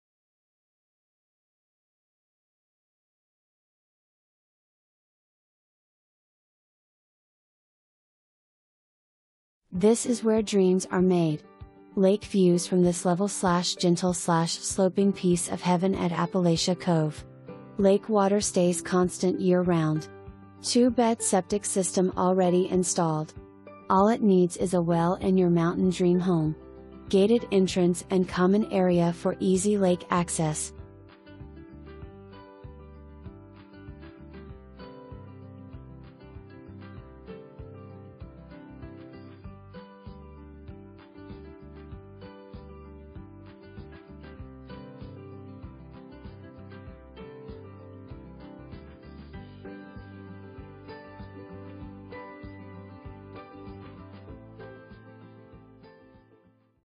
LT 27 APPALACHIA COVE
This is where dreams are made! Lake views from this level/gentle/sloping piece of heaven at Appalachia Cove. Lake water stays constant year-round. 2bed Septic system already installed. All it needs is a well and your mountain dream home! Gated entrance and common area for easy lake access.

Carol Robinson
Coldwell Banker High Country Realty